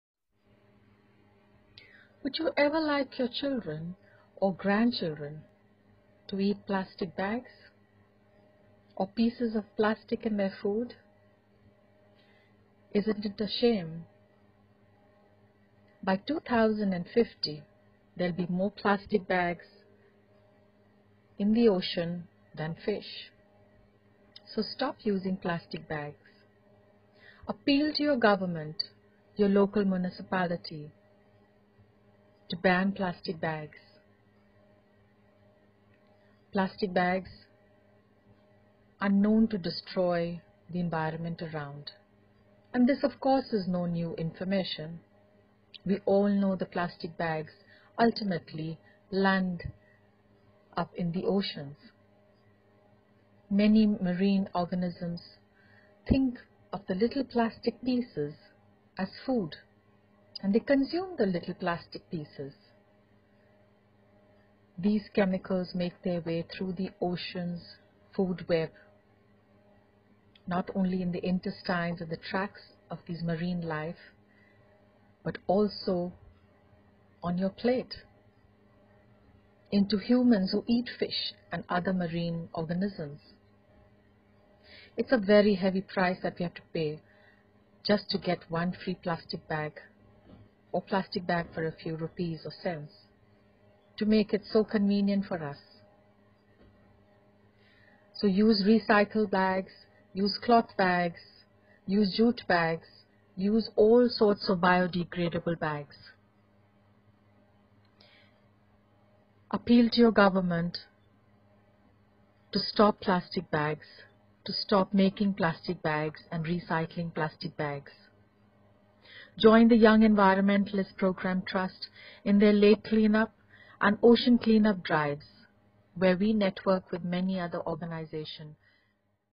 Ban Plastic bags in Mumbai Campaign by Young Environmentalists
Plastic bags take 500 to 1000 years to disintegrate. By 2050 there will be more plastic in the oceans than fish. Beside marine creatures our children may also end up eating plastics in the meals, refuse to use a plastic bag today. Join our cleanup projects. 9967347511